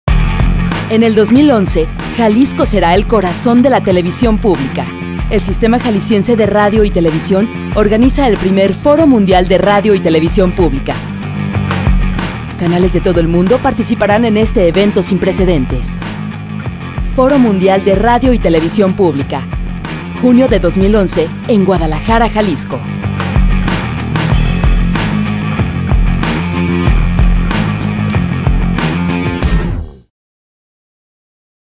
Foro Mundial de Medios Públicos
En junio de 2011, Guadalajara será sede de un evento sin precedente: el primero Foro Mundial de Medios Públicos, el cual reunirá a directores generales de televisoras y radiodifusoras públicas del mundo.